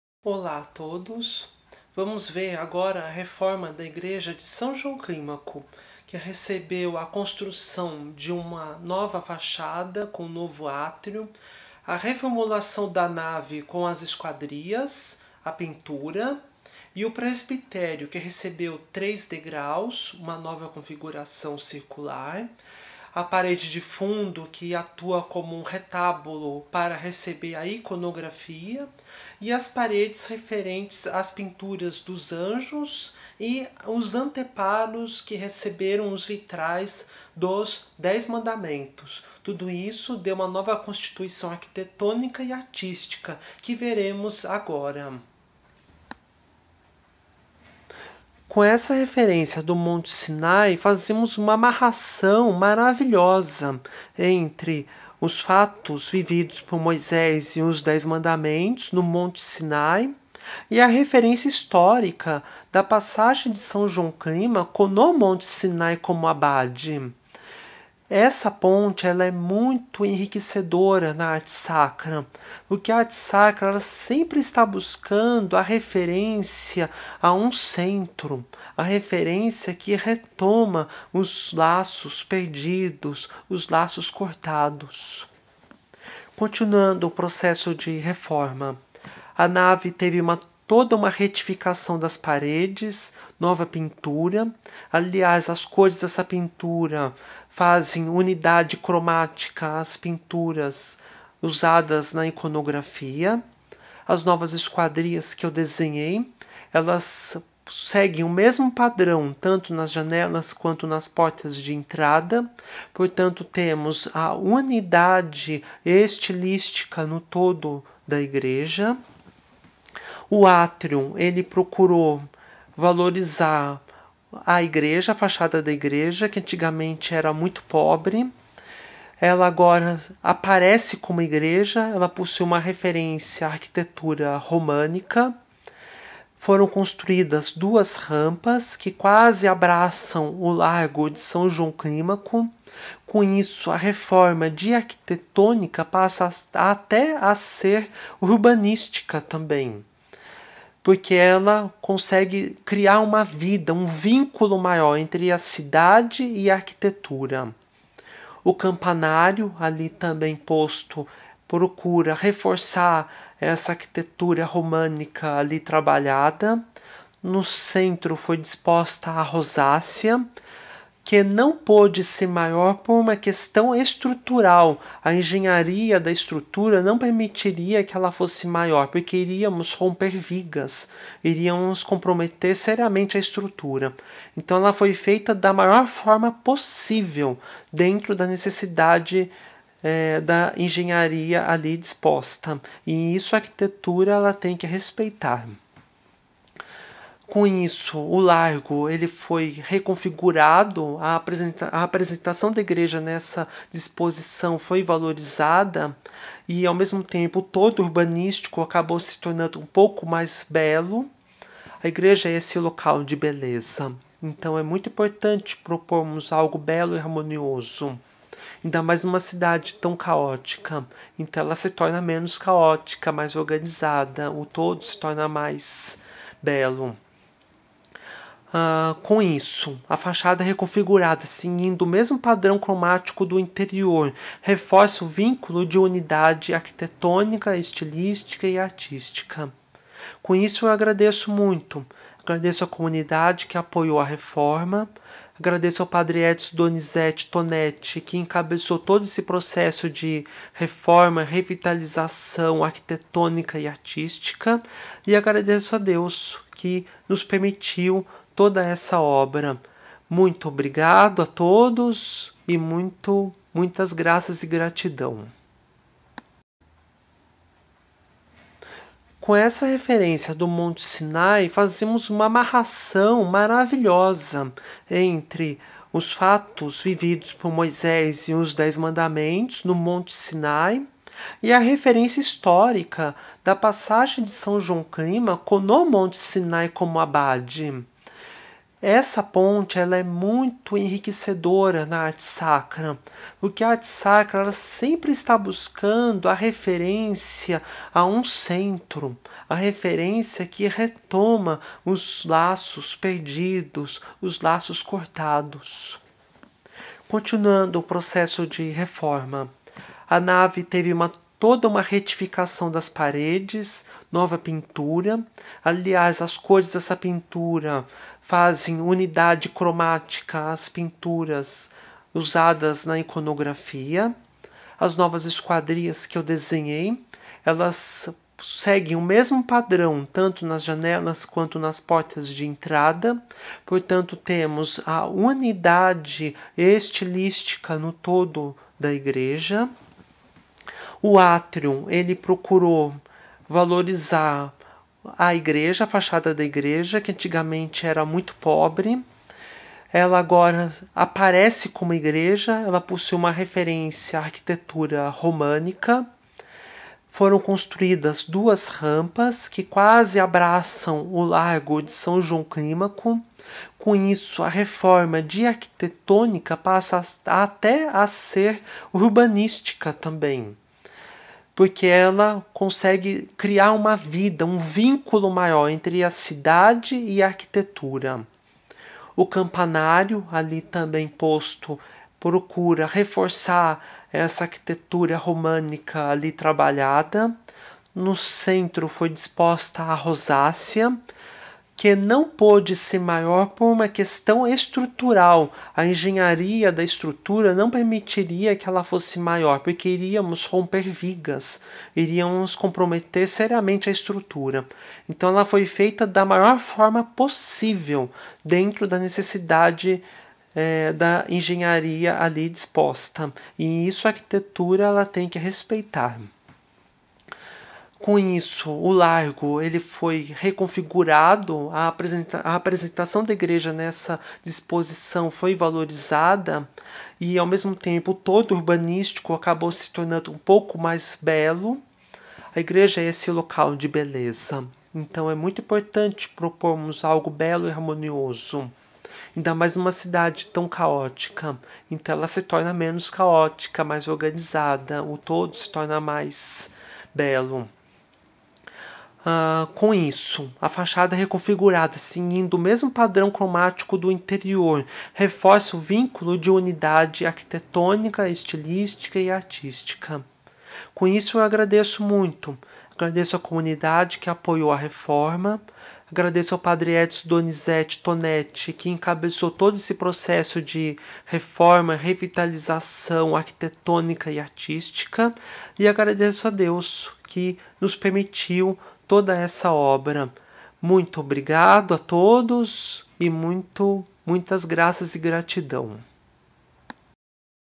São João Clímaco, arquitetura e arte sacra
Obra de revitalização arquitetônica e artística da igreja de São João Clímaco, em São Paulo, Região Ipiranga. São João Clímaco escreveu a obra A Escada Celeste, o Clímax em grego, escrita na época patrística esta obra foi o motor de inspiração da iconografia da obra da igreja. Foi construído um novo presbitério, o painel com as pinturas, os anjos, os vitrais com os Dez Mandamentos, a nova entrada da igreja e a nova entrada pelo largo de São João Clímaco. Obra realizada e apresentada por Marco Funchal.